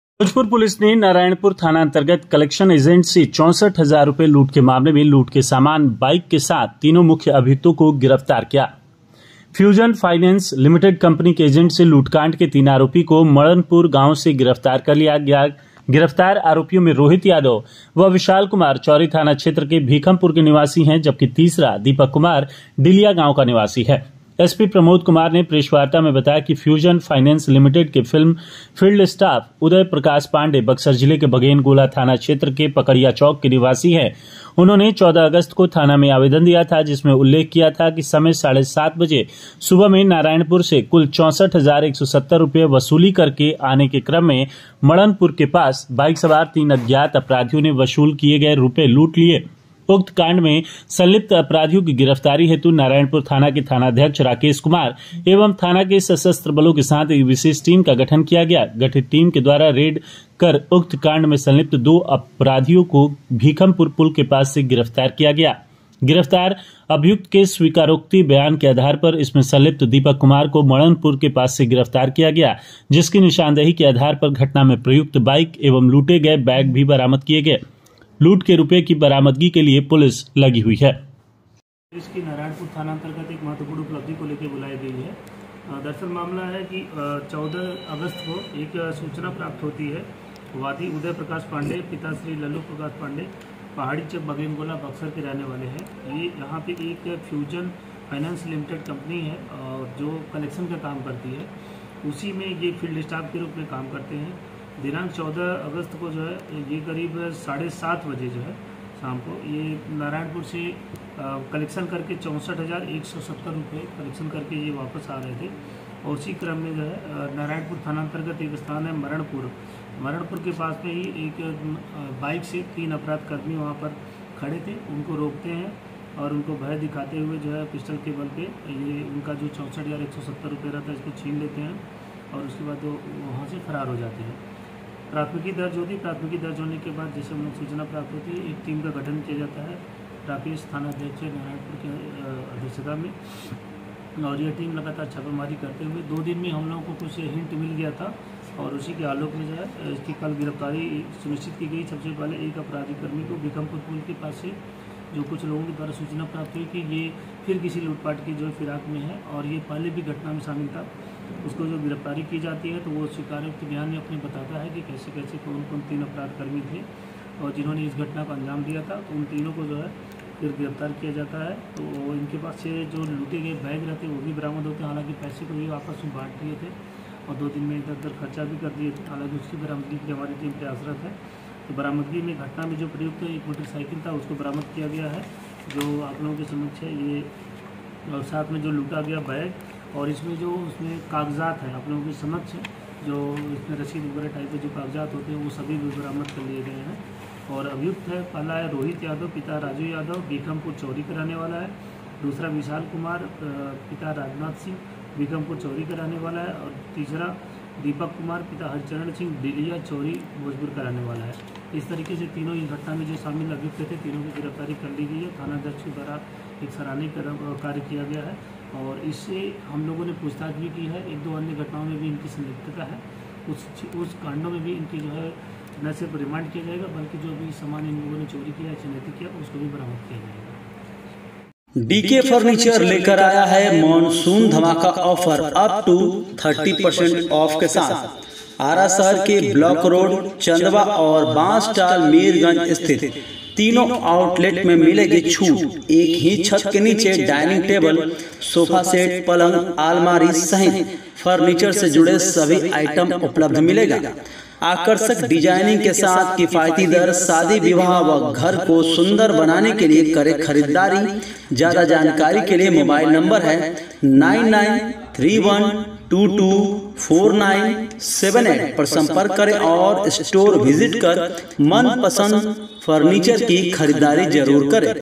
नारायणपुर थाना अंतर्गत कलेक्शन एजेंट से 64000 लूट के मामले में लूट के समान, बाइक के साथ तीन गिरफ्तार
भोजपुर पुलिस ने नारायणपुर थाना अंतर्गत फाइनेंस कंपनी के एजेंट से 64000 लूट के मामले में लूट के समान, बाइक के साथ तीनों मुख्य अभियुक्त को किया गिरफ्तार bihar ara bhojpur biharnewslive आरा भोजपुर aranews biharnews fourthpillar localnews @fourthpillarlive